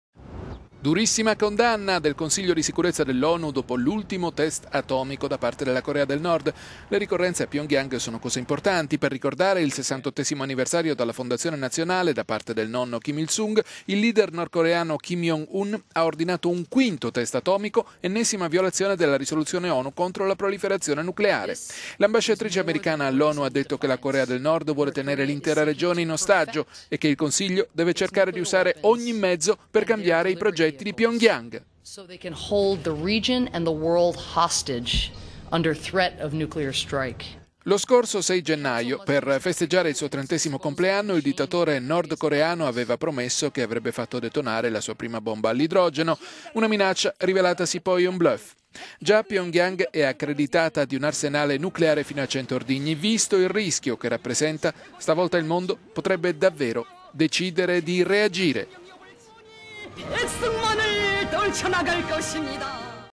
Dopo gli ultimi test nucleari nordcoreani, l'Onu promette ripercussioni
Durissima condanna del Consiglio di Sicurezza dell'Onu dopo l'ultimo test atomico da parte della Corea del nord.

Le ricorrenze a Pyongyang sono cose importanti. Per ricordare il 68° anniversario dalla fondazione nazionale, da parte del nonno Kim Il-sung, il leader nordcoreano Kim Jong-un ha ordinato un quinto test atomico, ennesima violazione della risoluzione Onu contro la proliferazione nucleare.

L'ambasciatrice americana all'Onu ha detto che "la Corea del nord vuole tenere l'intera ragion…